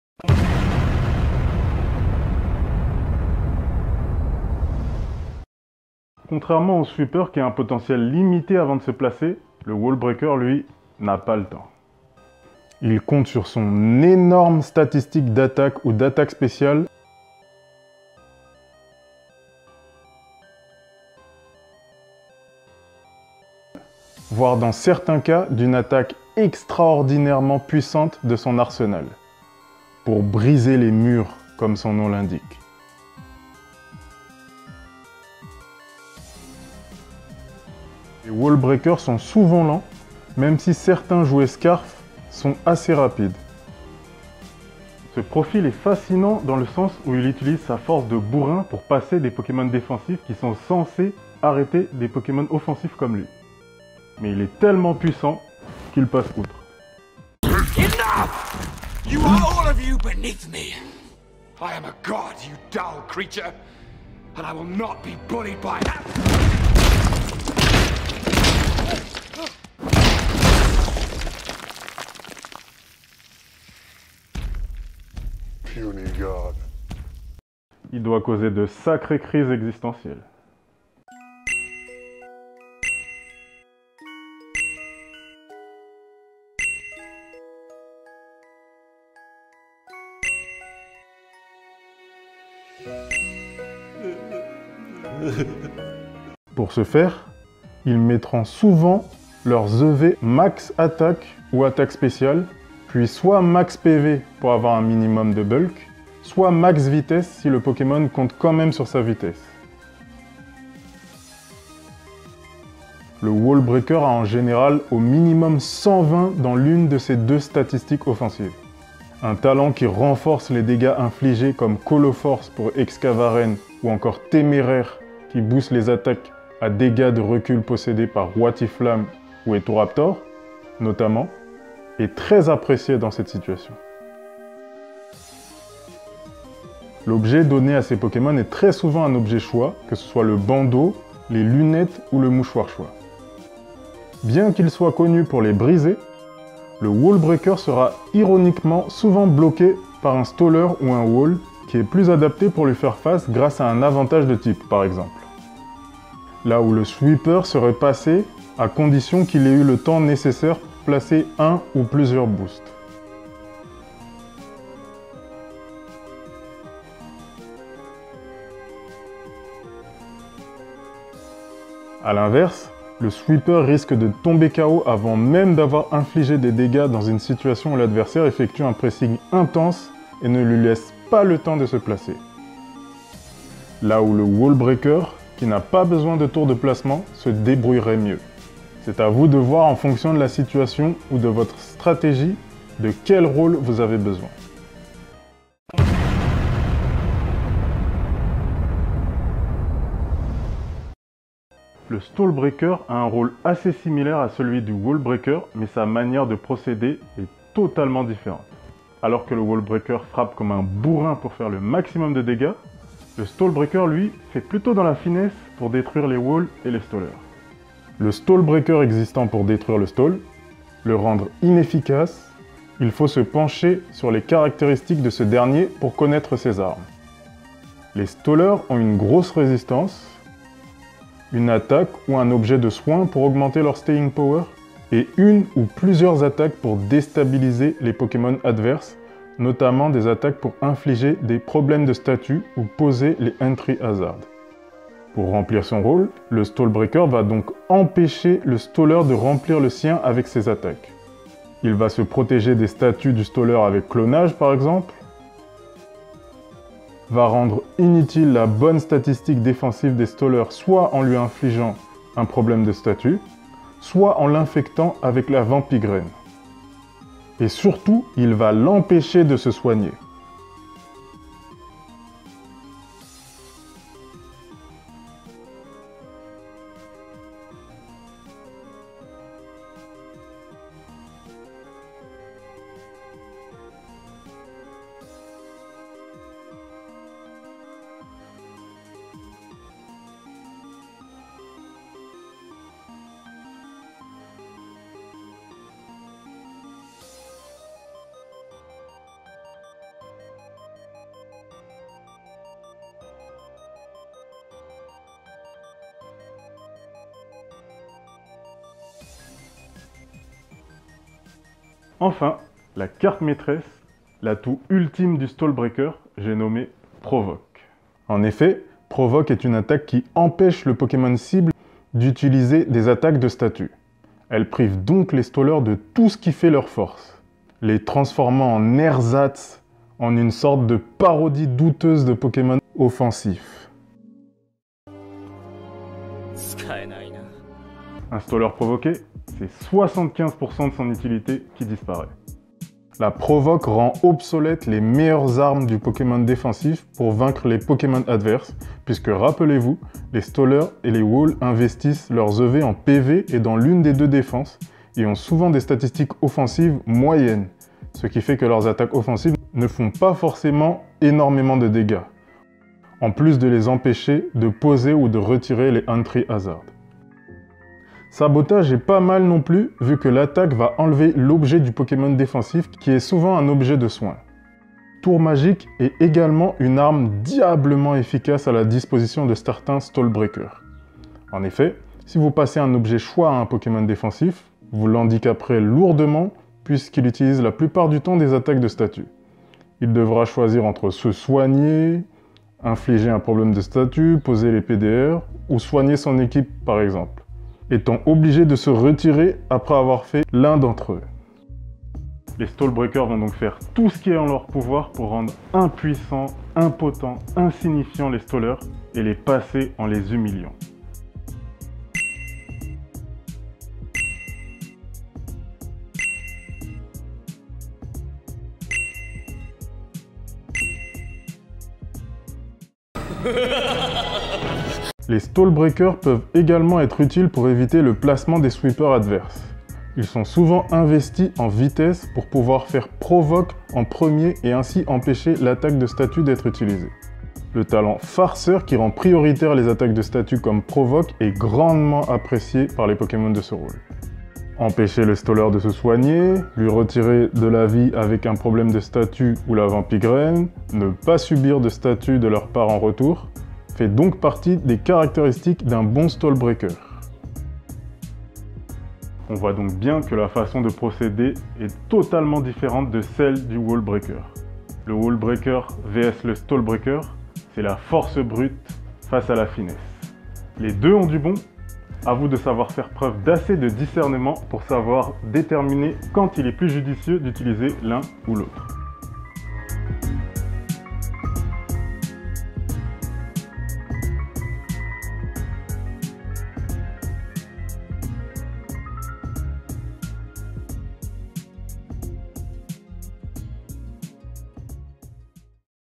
Les Rôles Pokémon Offensifs : Wallbreaker et Stallbreaker 3/6 [STRATÉGIE POKÉMON]
On poursuit notre série sur les différents offensifs que les pokémon peuvent remplir en combat pokémon stratégique en nous attardant cette fois-ci sur 2 rôles totalement opposés en ce qui concerne la manière de procéder : le Wallbreaker et le Stallbreaker. Bon visionnage.

0:00 Wallbreaker
3:15 Stallbreaker
7:24 Méthodes du stallbreaker